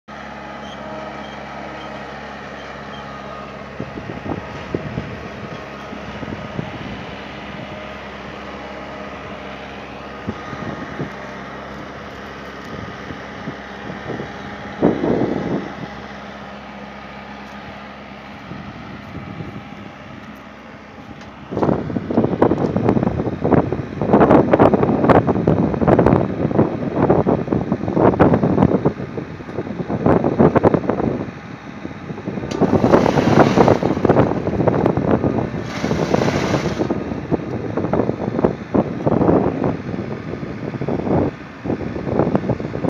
Badem Hasadı
Hasat Keşan a bağlı Karlı köyünde yapılmıştır. Ağaçlar 8 yaşında olup oldukça verimlidir. Günde 300 ağaca kadar hasat mümkündür.